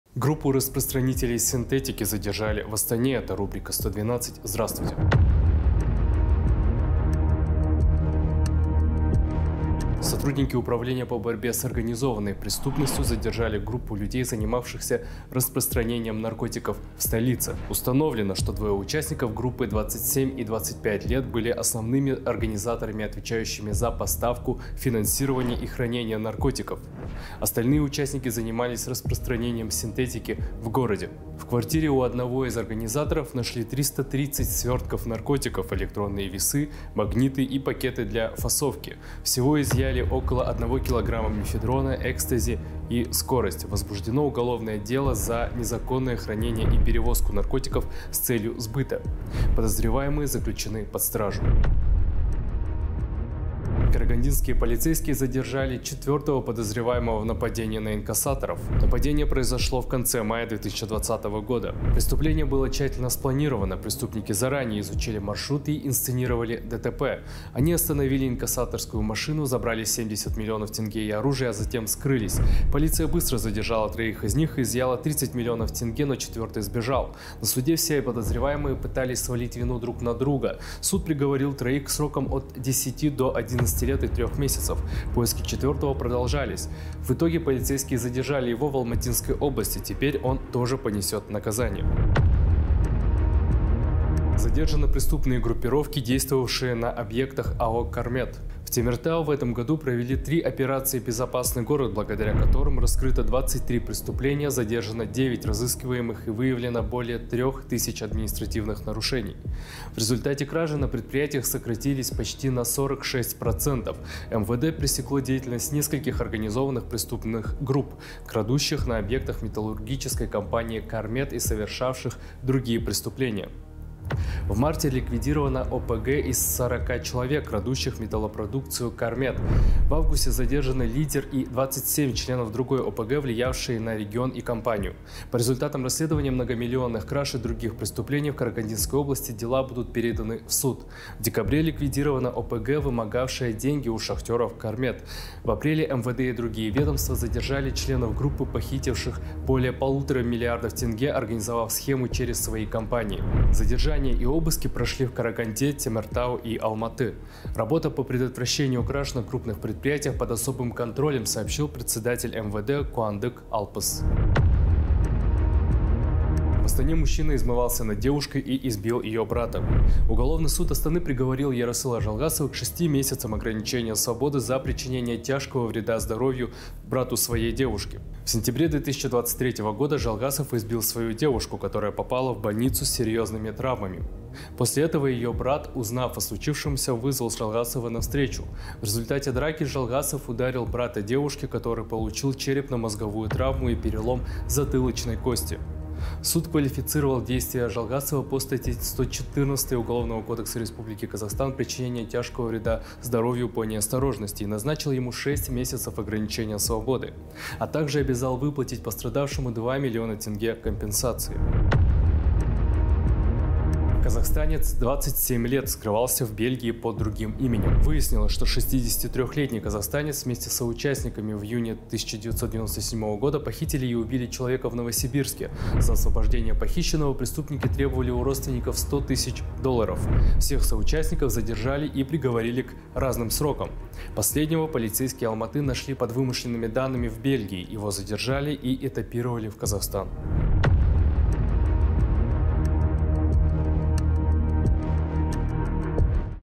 Задержание ОПГ и подозреваемого в нападении на инкасаторов
"112" - это рубрика, в которой рассказывается о громких и резонансных преступлениях и задержаниях за минувшую неделю.
El.kz Ақпараттық-танымдық портал